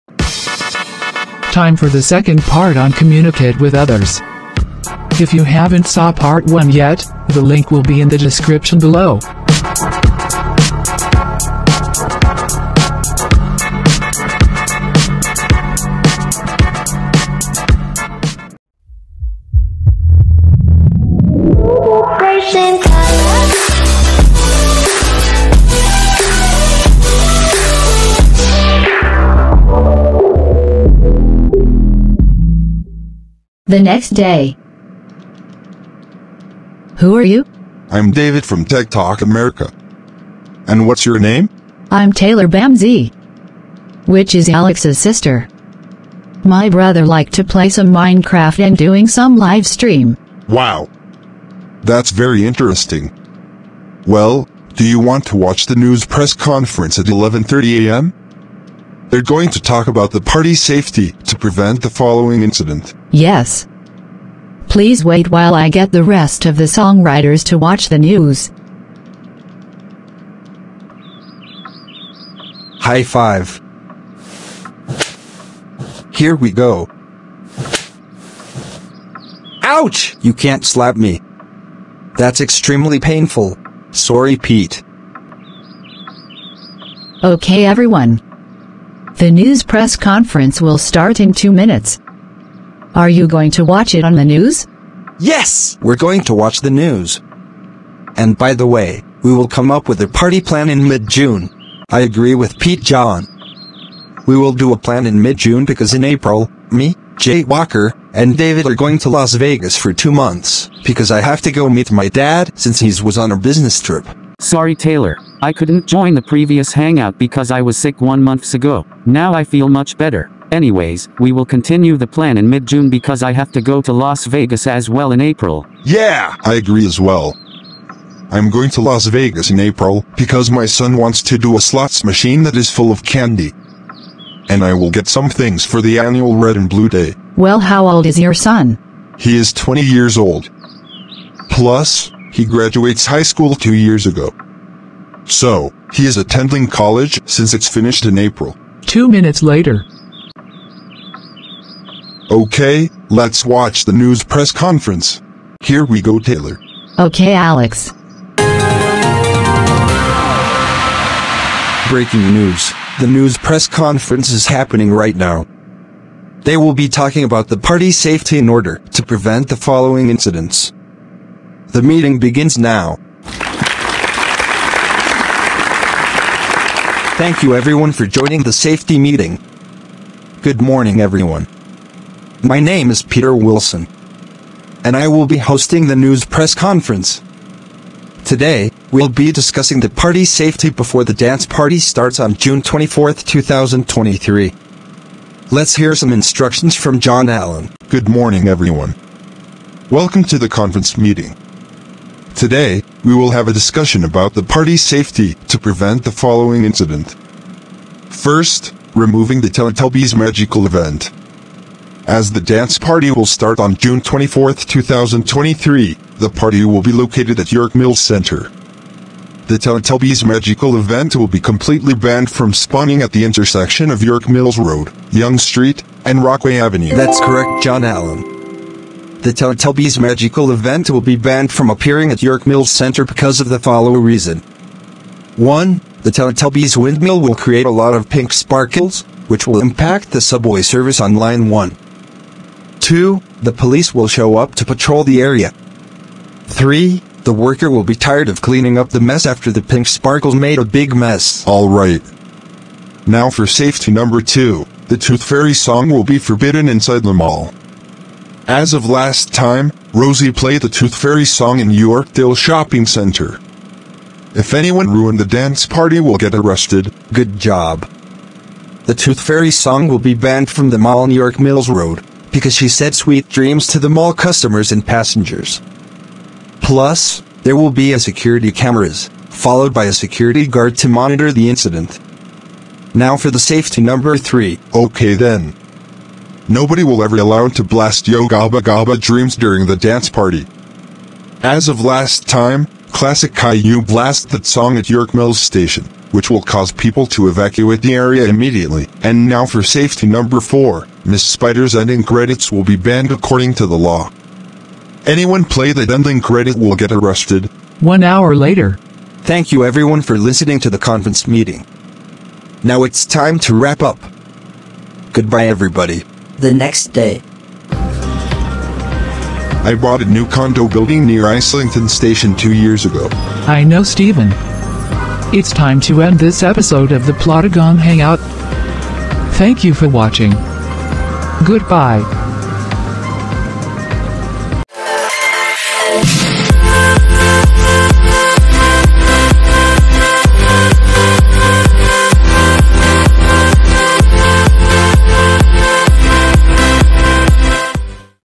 Communicate With Other (Not Quite A Saturday Show) | Plotagon Hangout Ep 4
The hangout will be premiere on June 16.

Since the dance party was approaching. I decided to post a party safety!

Plotagon Hangout is an animated virtual open ending discussion that talks about a topic, watches some TV,  or do something entertaining.